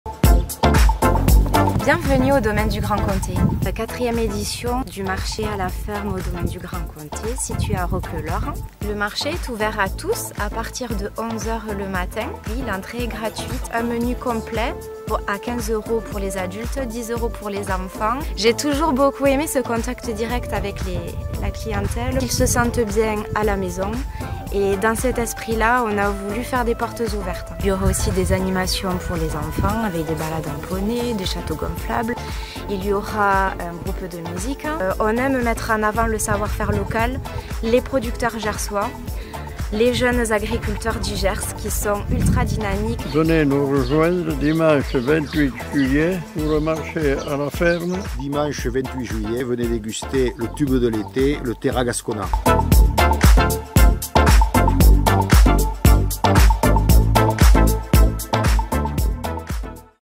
Marché au Domaine Du Grand Comté
[Marché au Domaine Du Grand Comté] Rencontre avec la famille Baylac, que je connaissais bien sur, de réputation, mais nous n'avions pas encore travaillé sur un projet communication. L'occasion s'est présentée par le biais d'un de mes clients Blé d'ici & Pain d'antan qui seront présent le Dimanche 28 juillet sur ce bien joli Domaine qui produit Vins et Armagnacs depuis 9 générations sur la commune de Roquelaure. Une belle idée de dimanche à la campagne ! parlez en à vos proches ! Ecoutons l'invitation des 3 générations de Baylac ! et merci pour ce partage pro 👍